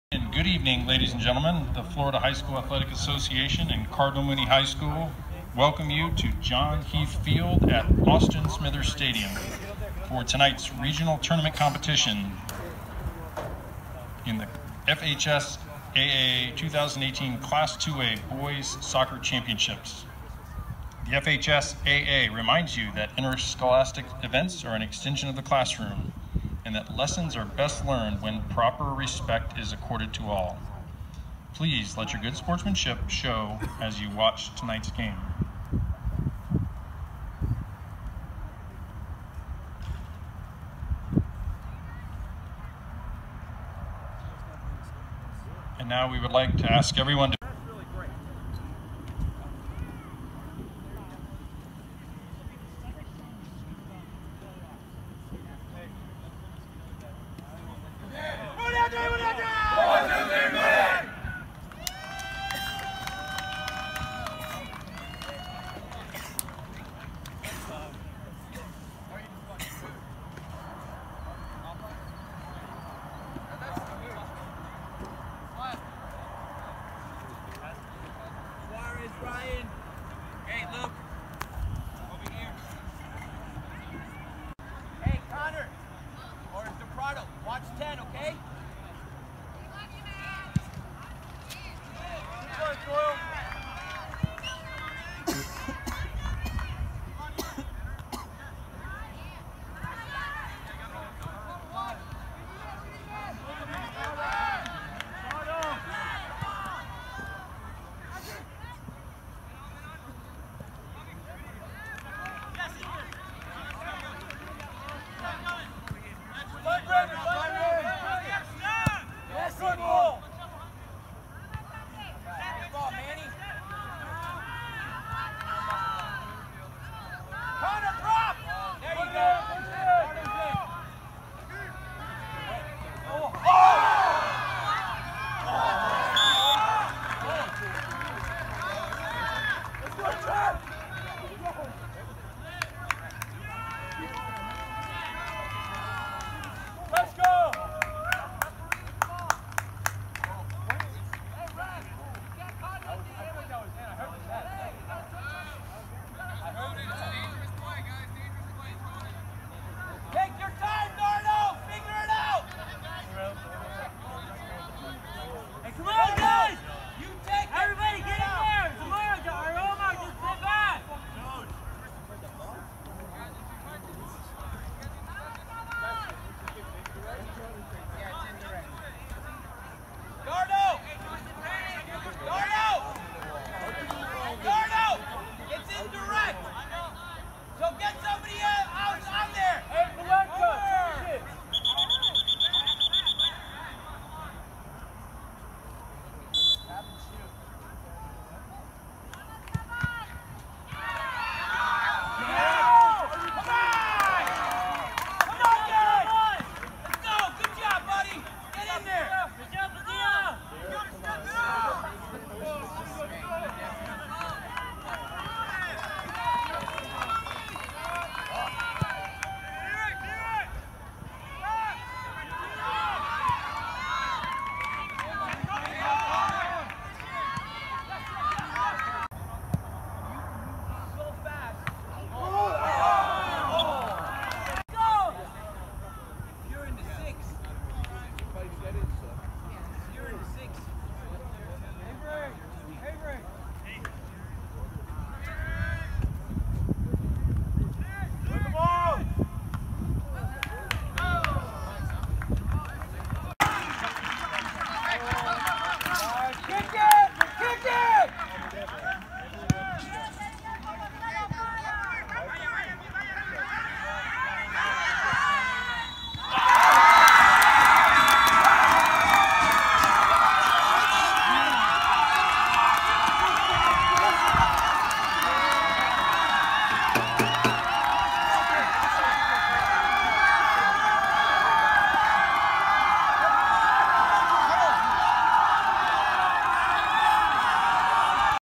Boys soccer: LaBelle 3, Cardinal Mooney 2
LaBelle ends Cardinal Mooney's season with a 3-2 victory in the Class 2A-Region 3 semifinals.